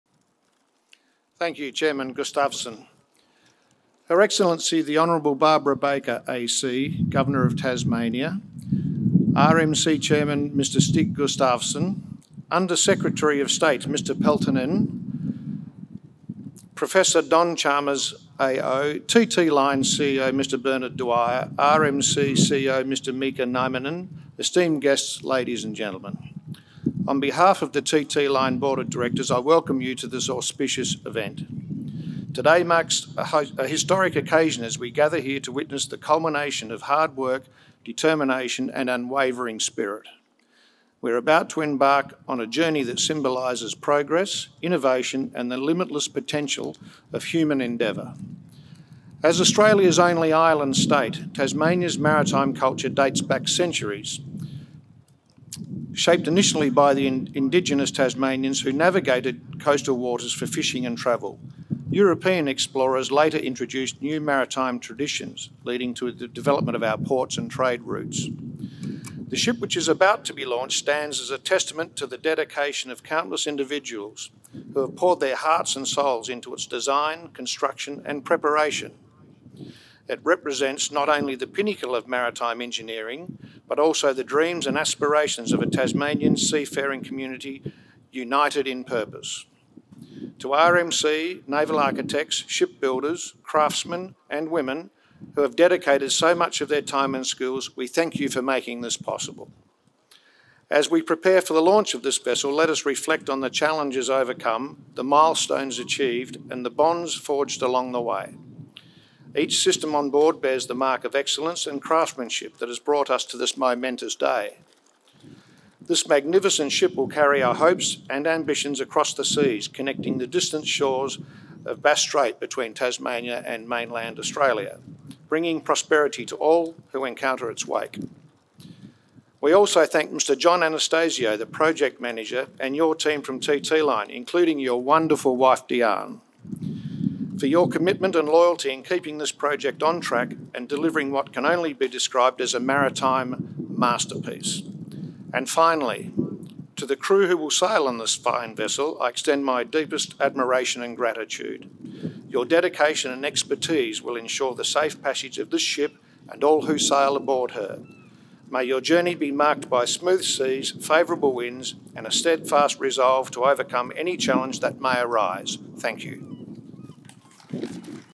Mike Grainger, Chairman of the Board of Spirit of Tasmania
Chairman of the Board of Spirit of Tasmania, Mike Grainger gave a speech at the launching ceremony of Spirit of Tasmania V on 19th July.